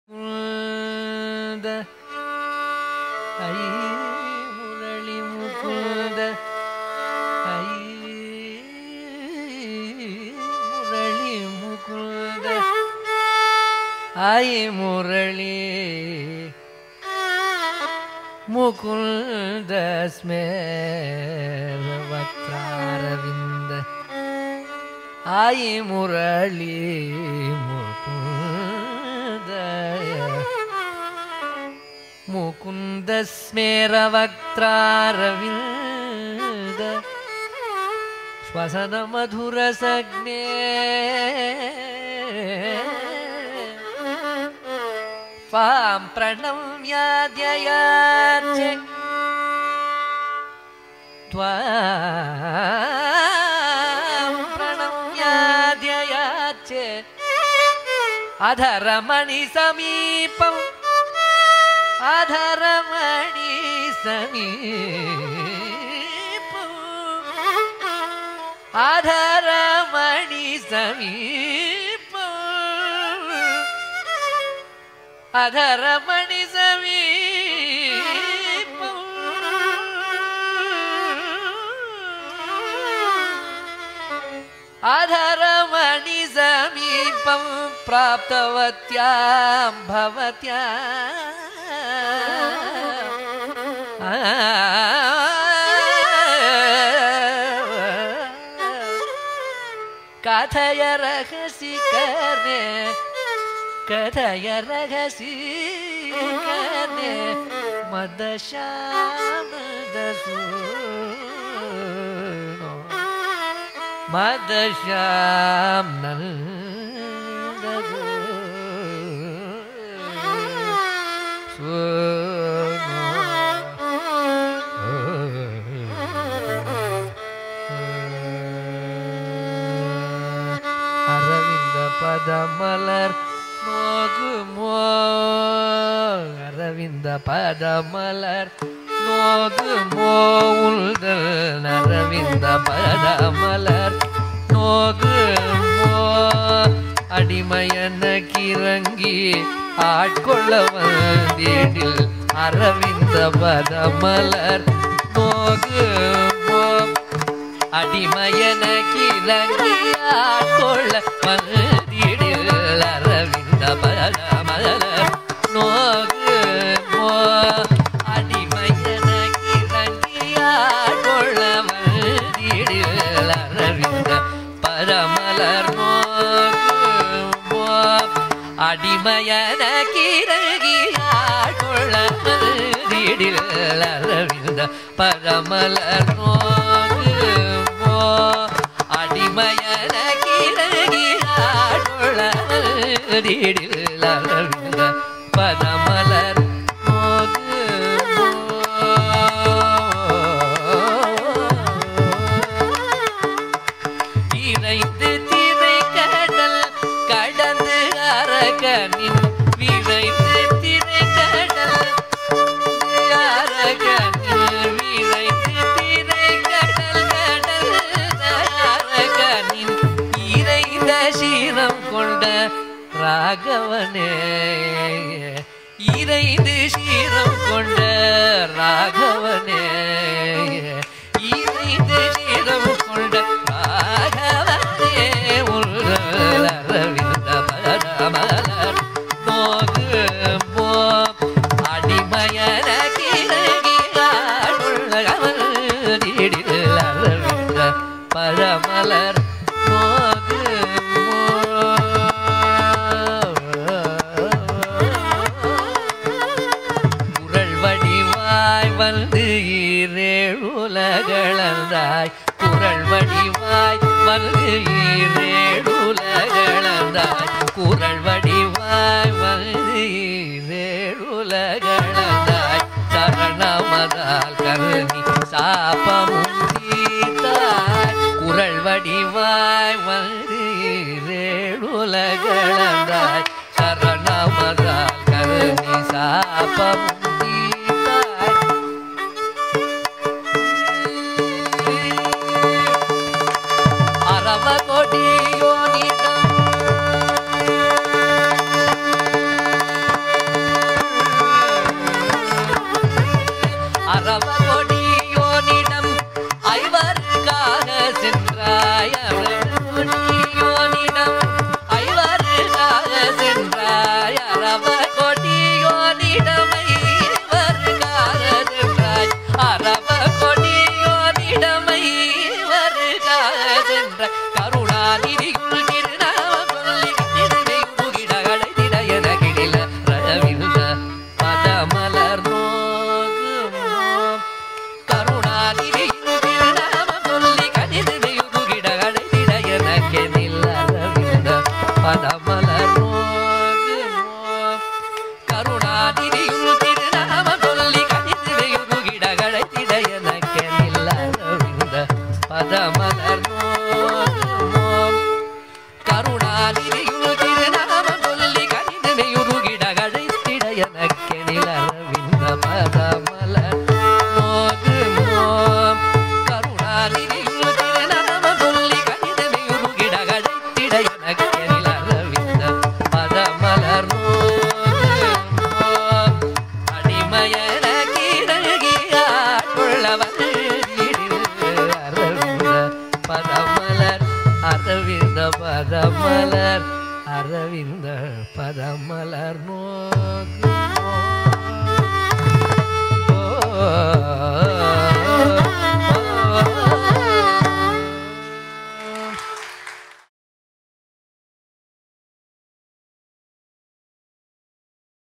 Sri. Ramakrishnan Murthy’s concert always leaves the rasika completely satisfied
Sri. Ramakrishnan Murthy’s concert always leaves the rasika completely satisfied having heard a wonderful concert by some of our great veterans in the field of Carnatic music.